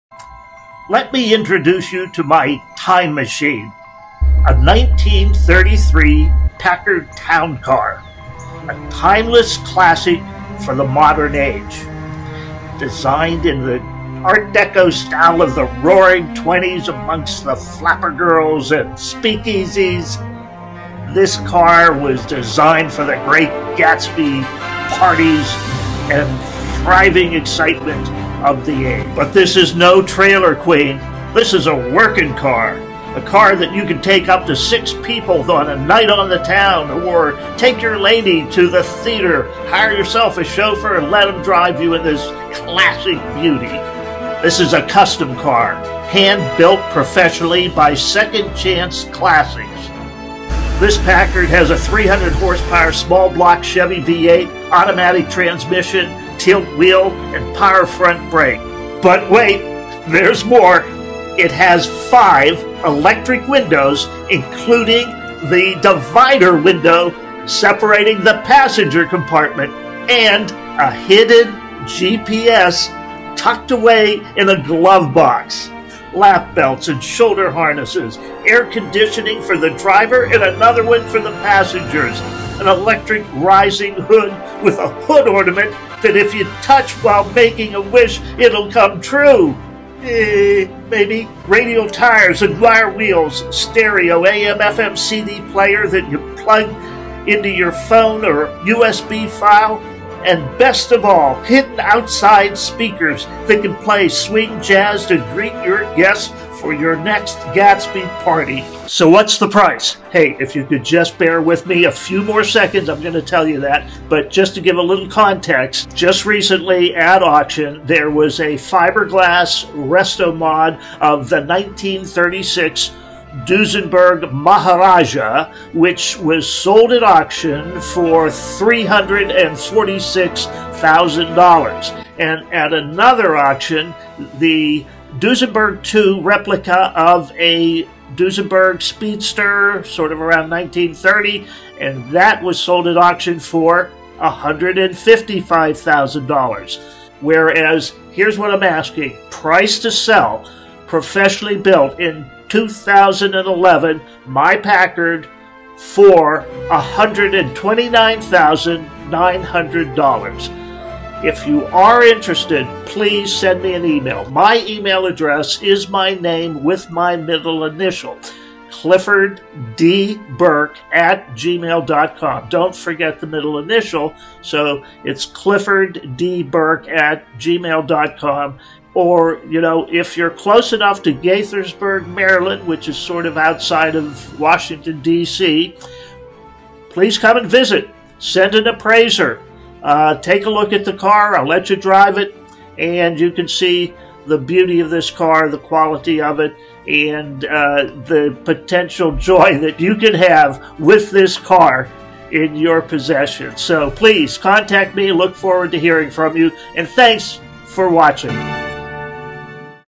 1933 Packard Limo for Sale in the "Great Gatsby" style for weddings, celebrations and pure fun.
1933 Packard Town Car for Sale as of July 2023. RARE, only one of six tribute 1933 Packard Town Car Limos meticulously molded by 2nd Chance Classics, Dwight Gibbons and built by his son Kyle Gibbons in 2011. Purpose built for limousine service so it can be driven on modern Interstates as well for its classic "Great Gatsby" style design.
This is a working car that can dress up for the opera or be the life of any blue collar party or event. Painted in classic "Tuxedo Black and Silver colors" this Packard is designed for easy maintenance and modern safety featuring seat and shoulder belts, tilt steering wheel, power steering, power disc brakes, A/C front and rear, electric windows with electric divider, two CD radios with hidden speakers that can be switched on to play "Swing Jazz" (or anything) outside of the Limo for AWESOME "GREAT GATSBY" PARTIES.
Highway speeds are powered by a GM 350 small block V-8, GM Turbo 3 speed Automatic and 9 inch Ford rear end. It can also wow a crowd with its electrically rising hood while a hidden electrical cutoff switch provides security. It even has a towing package with transmission cooler installed to tow a Marriott float in a parade.
This is a working car inspected twice each year which will be the "Crown Jewel" of any limo fleet or private owner as scene in VintageLimos.biz channel.